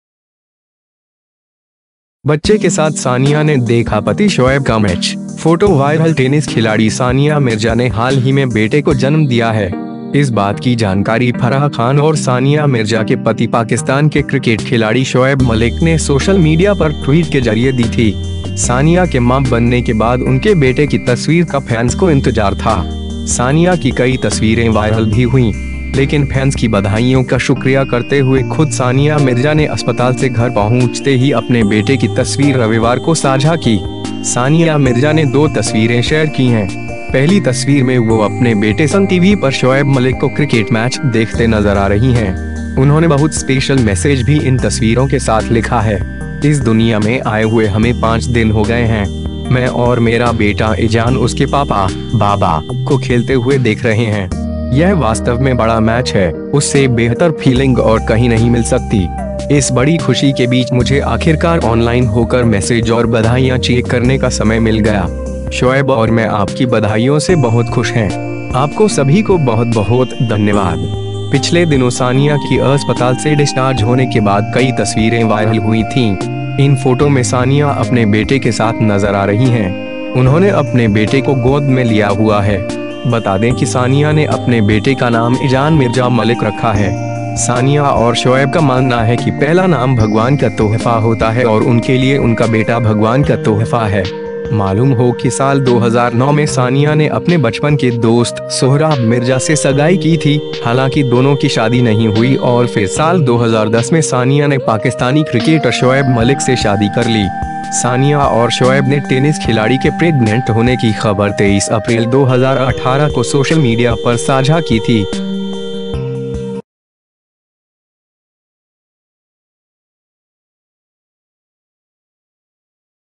बच्चे के साथ सानिया ने देखा पति शोएब का मैच, फोटो वायरल
टेनिस खिलाड़ी सानिया मिर्जा ने हाल ही में बेटे को जन्म दिया है. इस बात की जानकारी फराह खान और सान‍िया मिर्जा के पत‍ि पाकिस्तान के क्रिकेट खिलाड़ी शोएब मलिक ने सोशल मीडिया पर ट्वीट के जरिए दी थी. सान‍िया के मां बनने के बाद उनके बेटे की तस्वीर का फैंस को इंतजार था. सान‍िया की कई तस्वीरें वायरल भी हुईं. लेकिन फैंस की बधाईयों का शुक्र‍िया करते हुए खुद सान‍िया मिर्जा ने अस्पताल से घर पहुंचते ही अपने बेटे की तस्वीर रव‍िवार को साझा की.

सान‍िया मिर्जा ने दो तस्वीरें शेयर की हैं. पहली तस्वीर में वो अपने बेटे संग टीवी पर शोएब मल‍िक को क्र‍िकेट मैच देखते नजर आ रही हैं. उन्होंने बहुत स्पेशल मैसेज भी इन तस्वीरों के साथ ल‍िखा है.

"इस दुनिया में आए हुए हमें 5 दिन हो गए हैं.. मैं और मेरा बेटा इजान उसके पापा (बाबा) को खेलते हुए देख रहे हैं. यह वास्तव में बड़ा मैच है, उससे बेहतर फीलिंग और कहीं नहीं मिल सकती. इस बड़ी खुशी के बीच मुझे आखिरकार ऑनलाइन होकर मैसेज और बधाइयां चेक करने का समय मिल गया. शोएब और मैं आपकी बधाइयों से बहुत खुश हैं. आपको सभी को बहुत-बहुत धन्यवाद."

पिछले दिनों सान‍िया की अस्पताल से ड‍िस्चार्ज होने के बाद कई तस्वीरें वायरल हुई थीं. इन फोटो में सानिया अपने बेटे के साथ नजर आ रही हैं. उन्होंने अपने बेटे को गोद में लिया हुआ है. बता दें कि सानिया ने अपने बेटे का नाम इजान मिर्जा मलिक रखा है.

सानिया और शोएब का मानना है कि पहला नाम भगवान का तोहफा होता है और उनके लिए उनका बेटा भगवान का तोहफा है.

मालूम हो कि साल 2009 में सानिया ने अपने बचपन के दोस्त सोहराब मिर्जा से सगाई की थी, हालांकि दोनों की शादी नहीं हुई और फिर साल 2010 में सानिया ने पाकिस्तानी क्रिकेटर शोएब मलिक से शादी कर ली. सानिया और शोएब ने टेनिस खिलाड़ी के प्रेग्नेंट होने की खबर 23 अप्रैल 2018 को सोशल मीडिया पर साझा की थी.